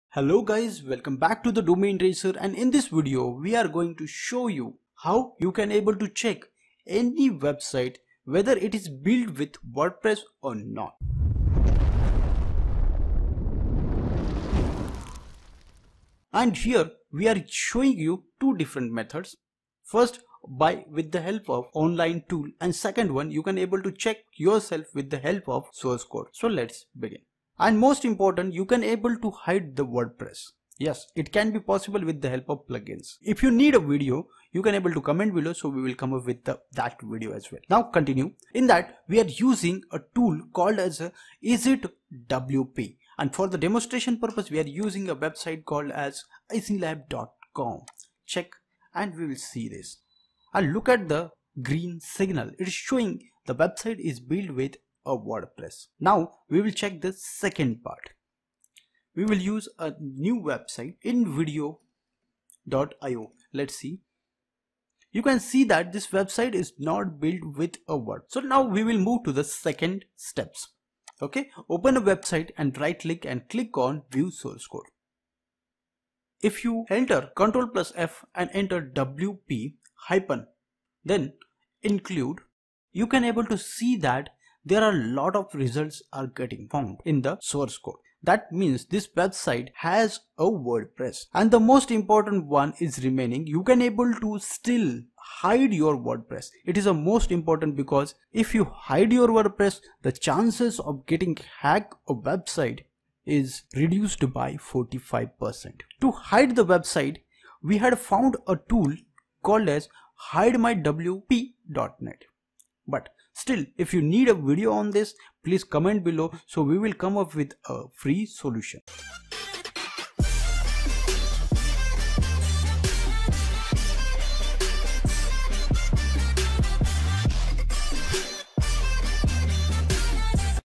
How To Check If A Website Is Built On WordPress // Website Detector {2025}
Do Want to know if a website is made with WordPress or not, then this video will be very helpful for you. In this video tutorial, we will teach you how to check if a website is built on WordPress.
We will share with you two methods to find out if a website is built on WordPress or not. The first method is to check my WordPress website checker tool and the second one is with the help of source code.
So, watch this complete video to use these two unique methods to know if a website is made with WordPress.